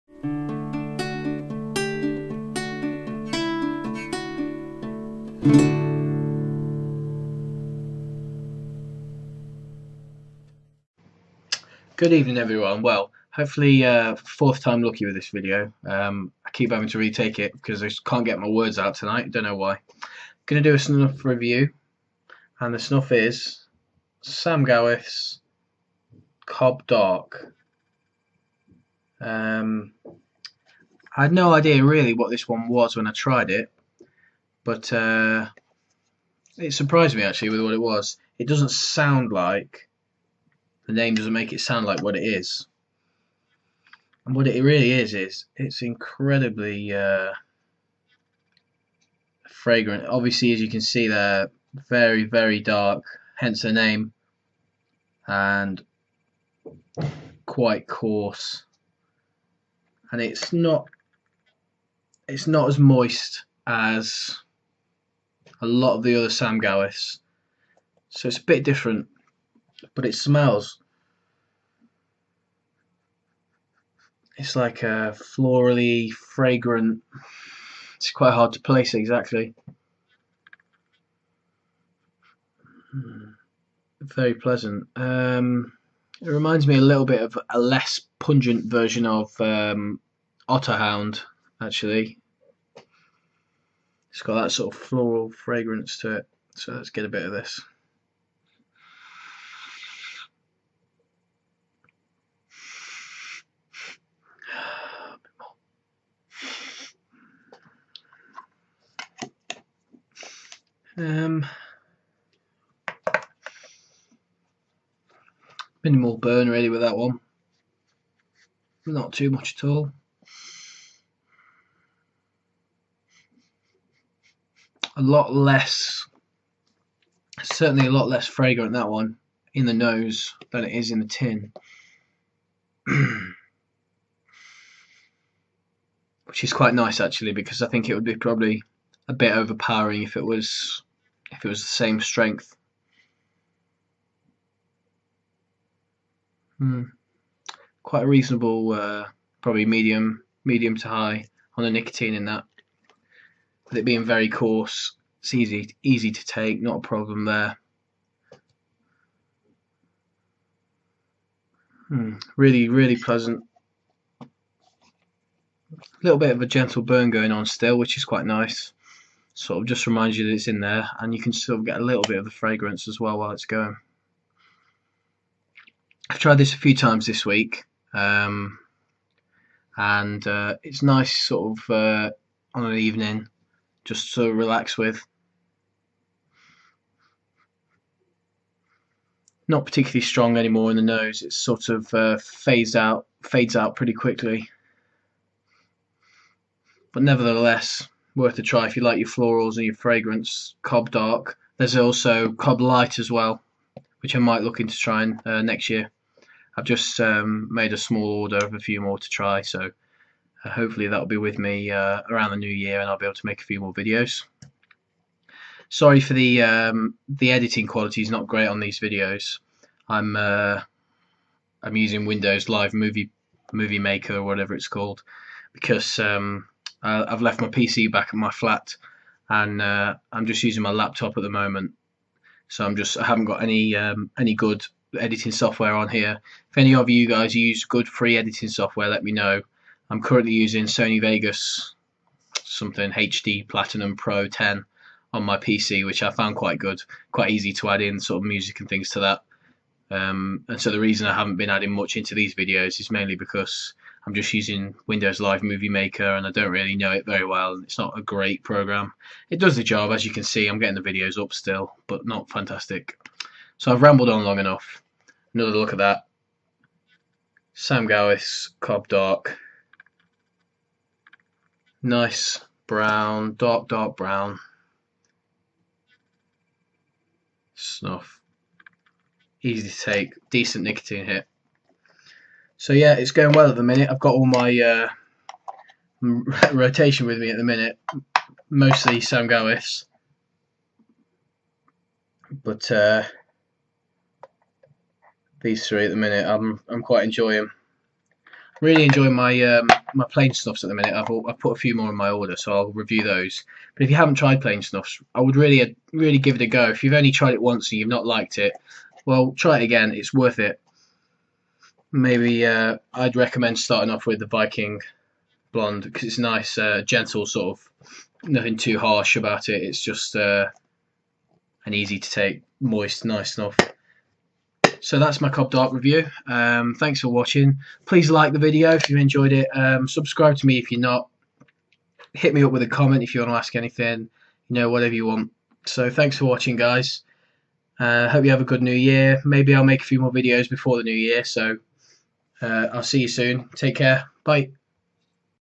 Samuel Gawith Cob Dark - Nasal Snuff Review
Interesting snuff, quite like it :)

Cheers, SB